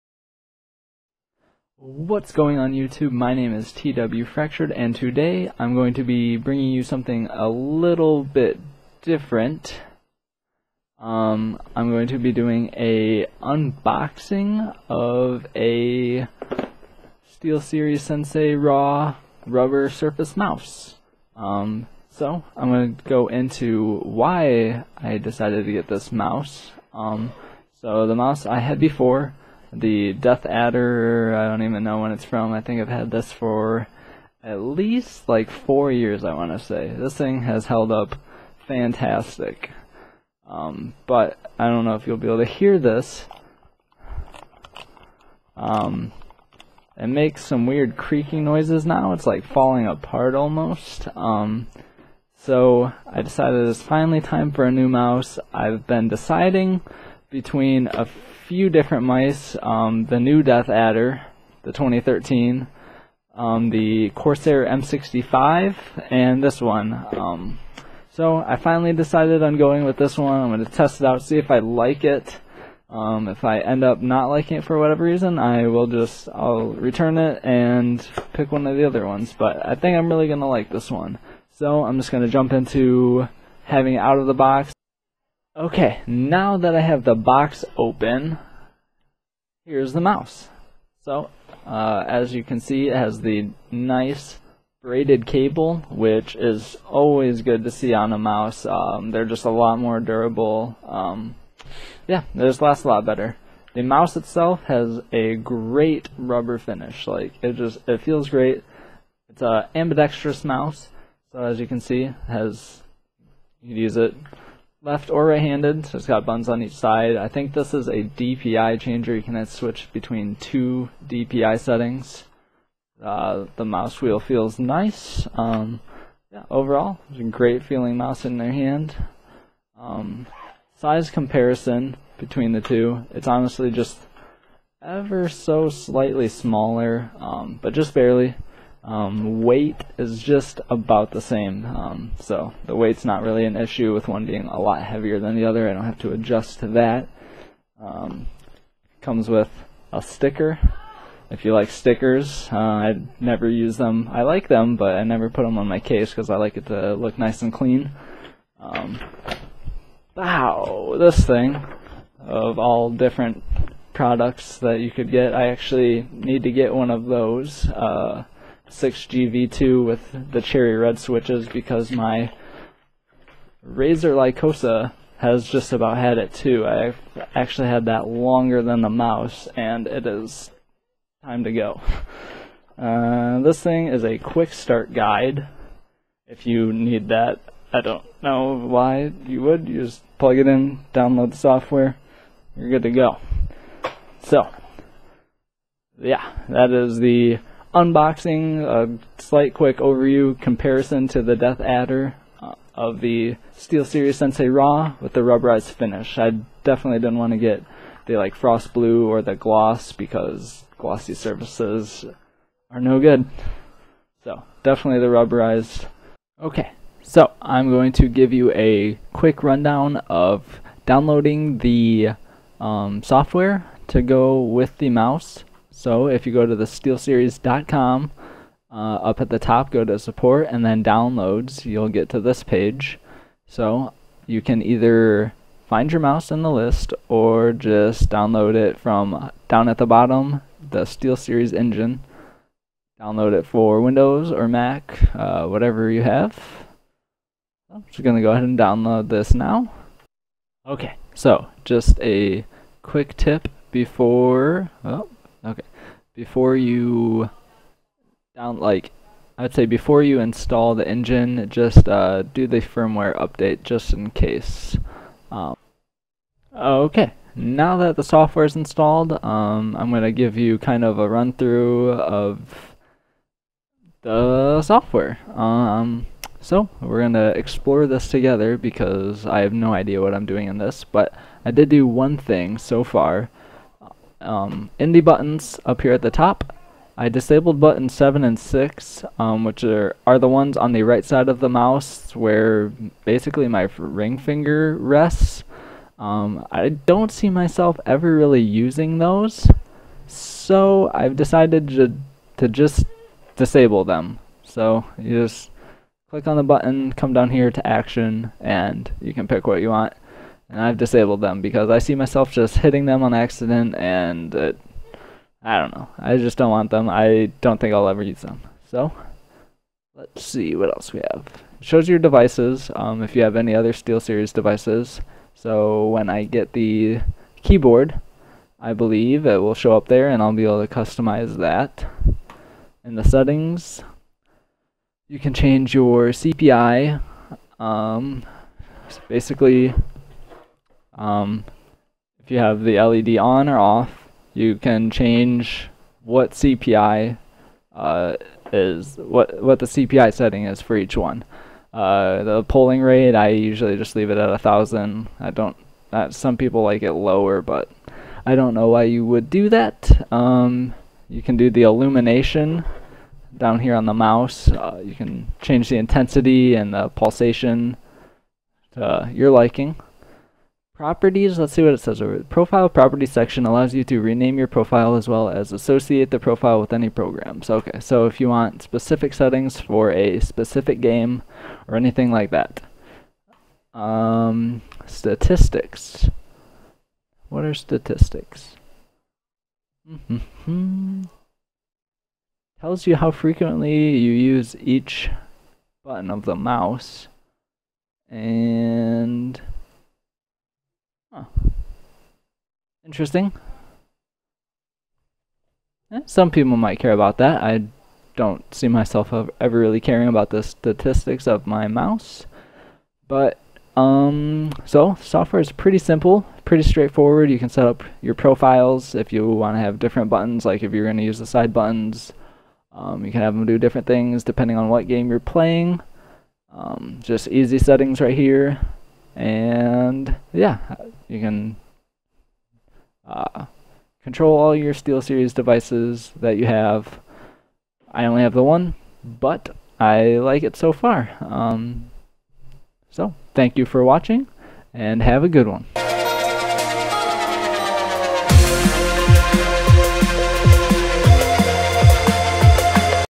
SteelSeries Sensei Raw Unboxing/Personal Comparison to Razer Deathadder/Quick software walkthrough
My first ever unboxing video, so I would certainly appreciate any tips.  I am unboxing the SteelSeries Sensei Raw gaming mouse.  I do a quick personal comparison to the Razer Deathadder which I was previously using.  Then I runthrough the SteelSeries Engine software.